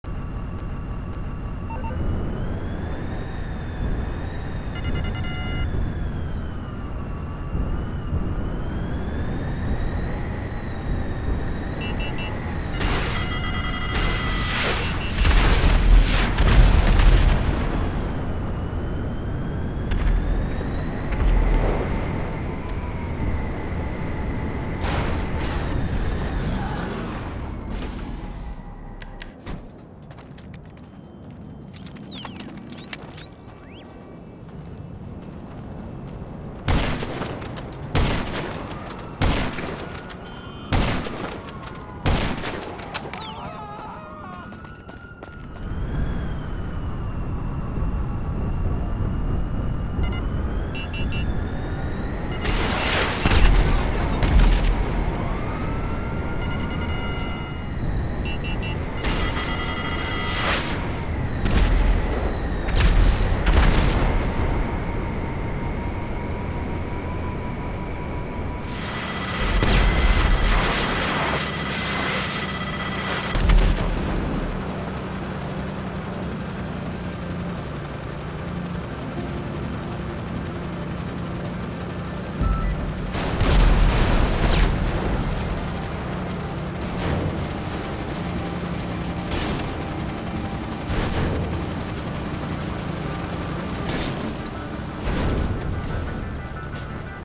Humiliating an Oppressor MK2 griefer with the Deluxo
Whenever tryhards are getting demolished they always want to do a 1v1 at the beach because they're only good at sniping lol